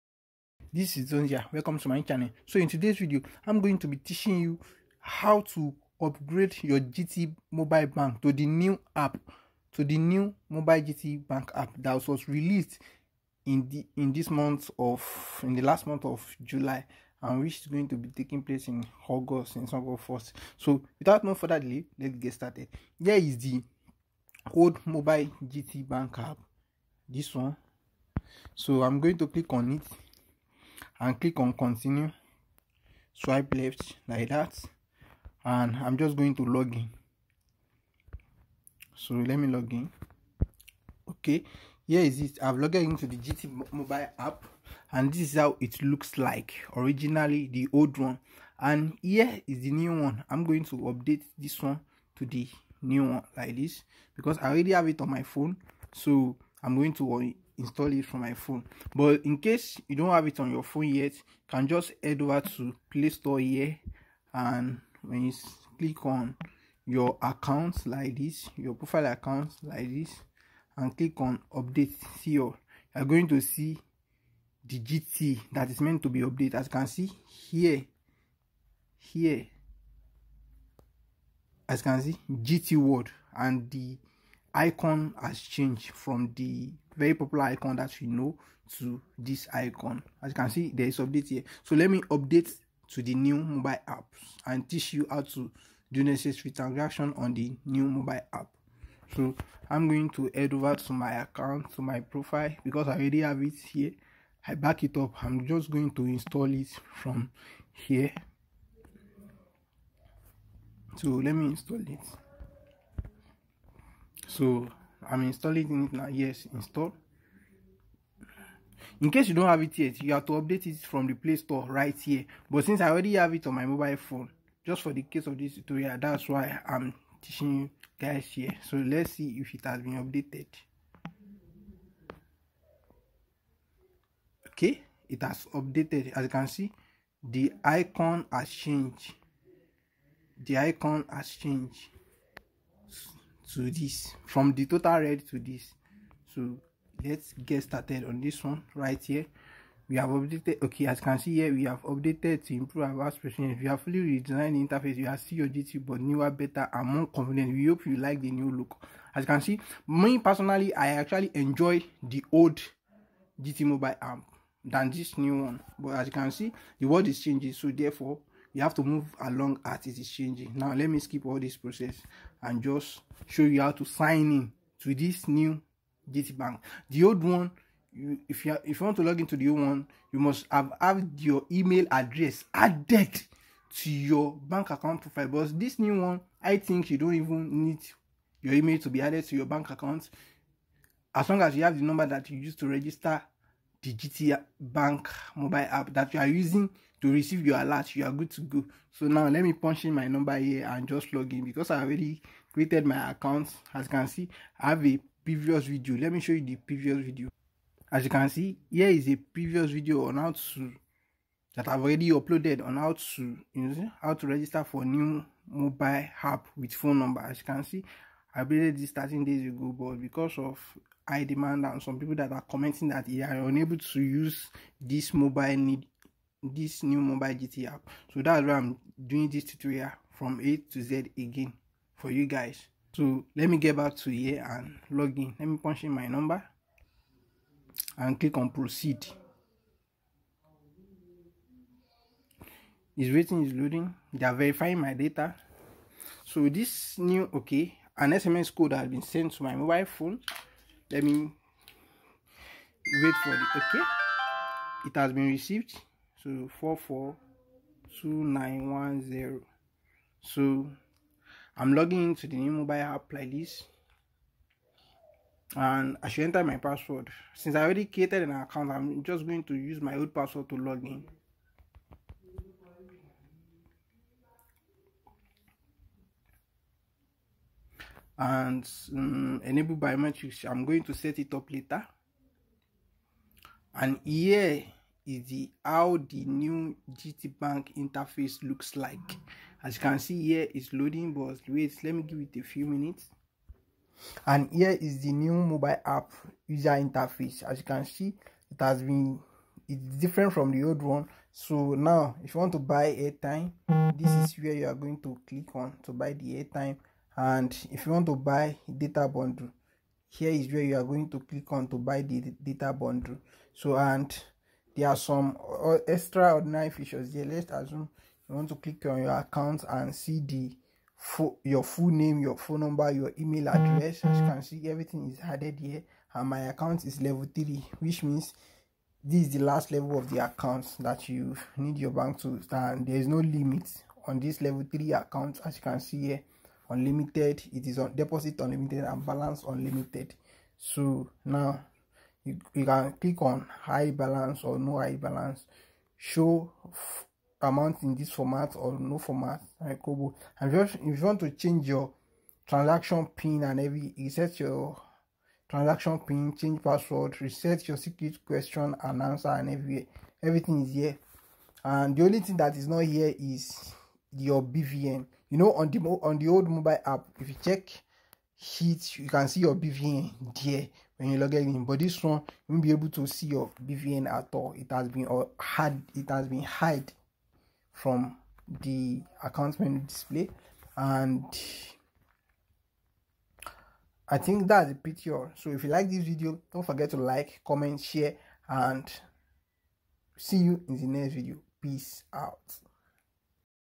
NEW UPDATE: GTBANK OLD MOBILE APP VS NEW GTBANK MOBILE APP IS IT WORTH UPGRADING?
In This Video, I'm Going To Teach You A Step By Step Tutorial On How To On How UPGRADE Your Gtbank internet mobile app